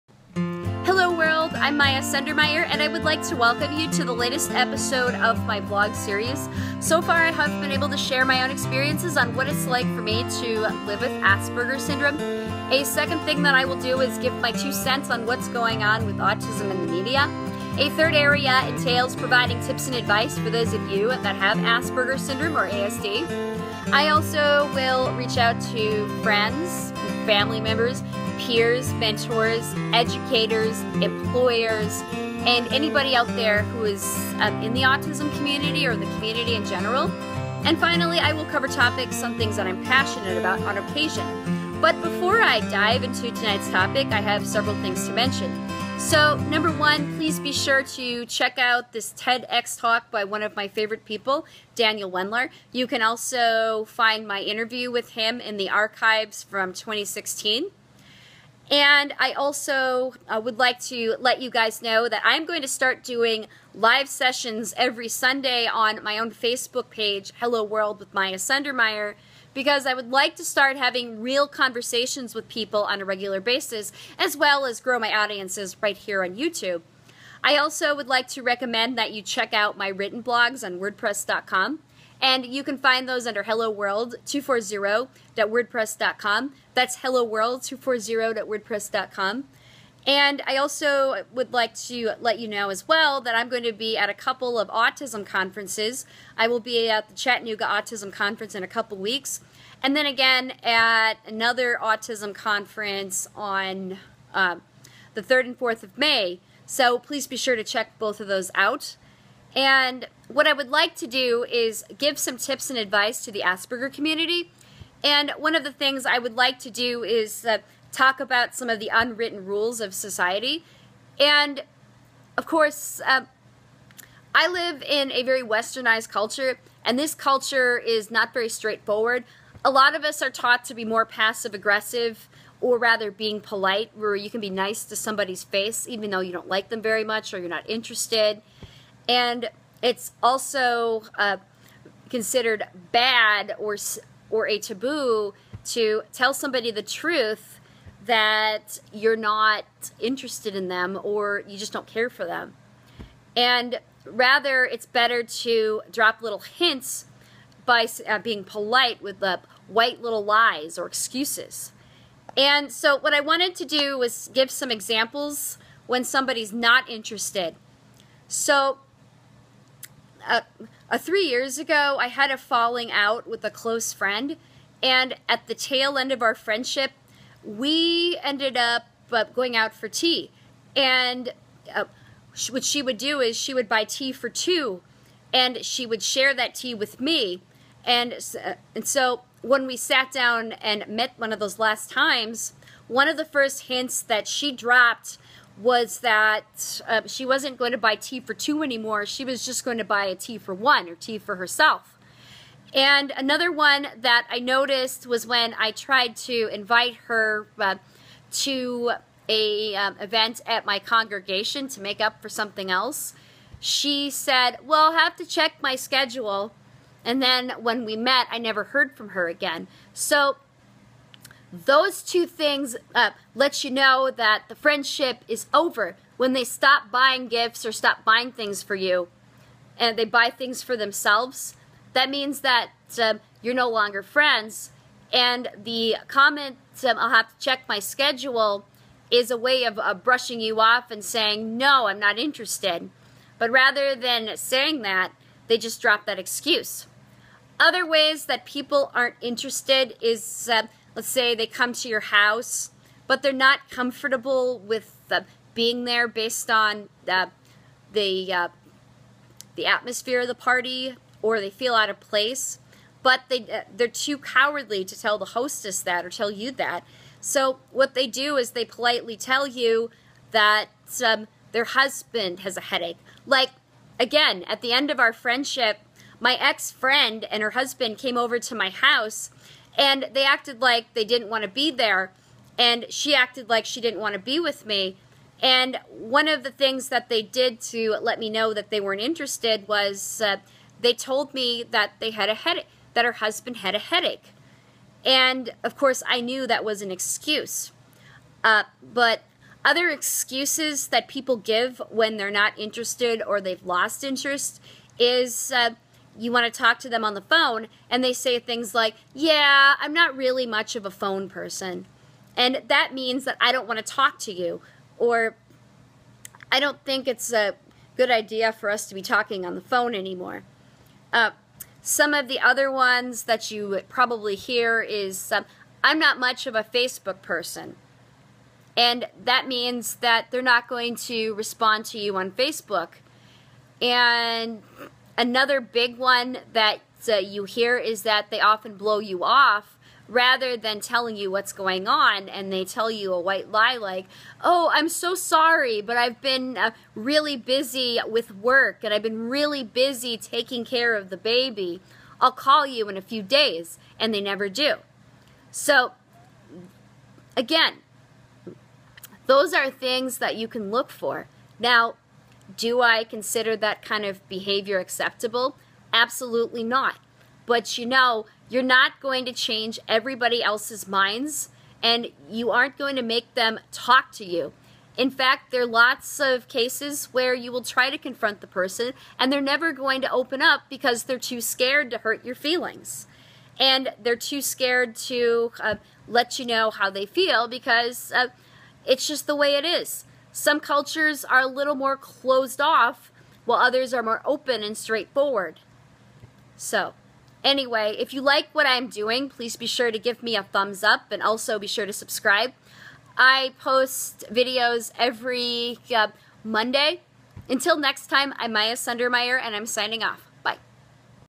How to look for signs when other people aren't interested in you.
For one on the spectrum, reading between the lines and understanding the concept of unwritten social rules is blurry and hard to follow.  One of the things that we struggle with is really picking up on hints when someone is not interested in us.  So, I provide tips and advice on looking for signs that someone is not interested in us.